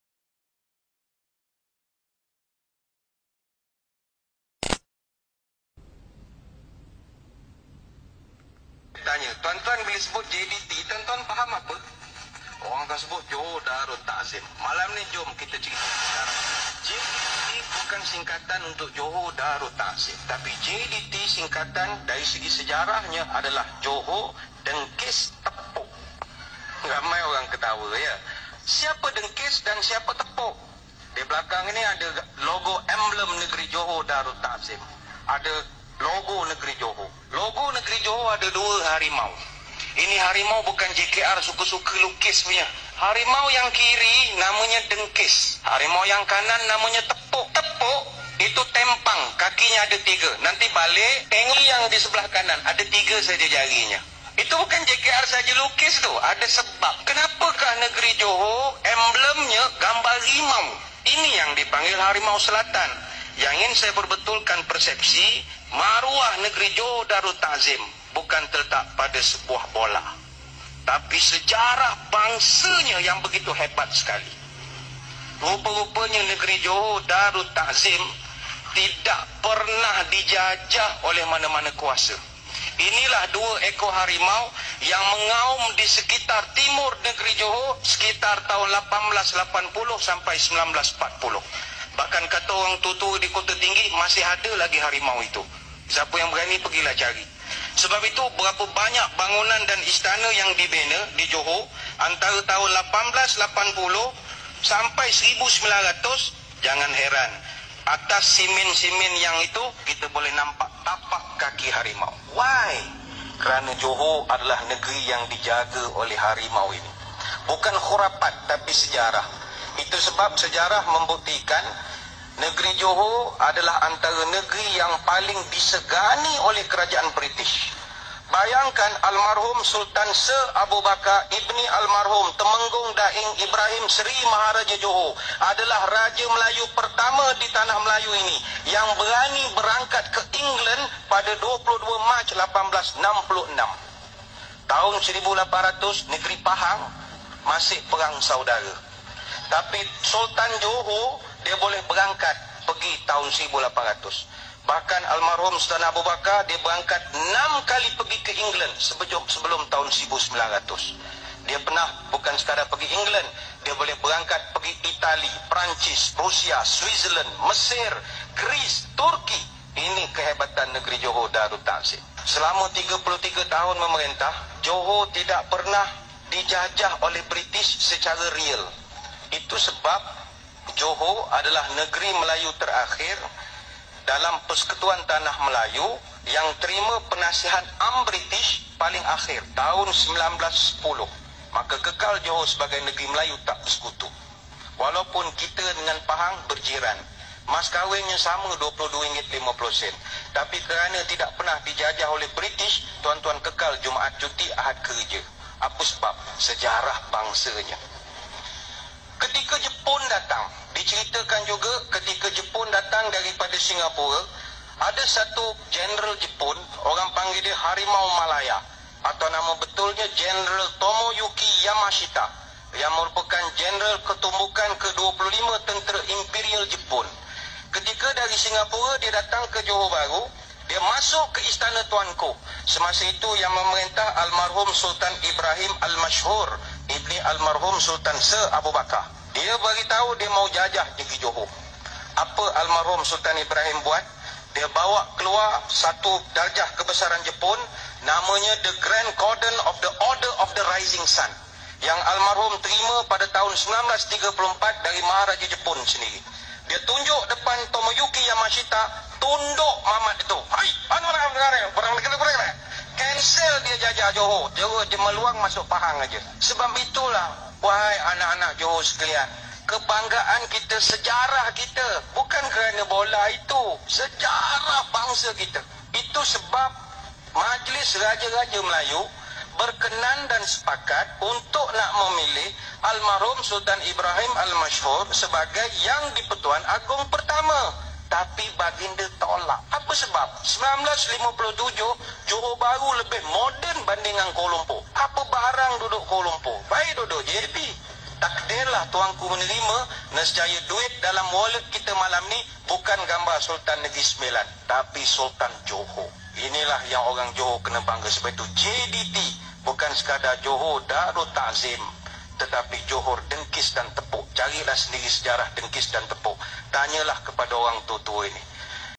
Maksud JDT Johor Dengkis Tepok
Sejarah Johor Bukan Dari Kehebatan Bola Sepak, Tetapi Lebih Dari Itu. Bermula Dari Sejarah Sultan Johor Tahun 1800, Dengkis Tepok, Negeri Terakhir Terima Sistem Perintahan Penjajah, Ditawarkan Sultan Johor Jadi Yang Dipertuan Agong Pertama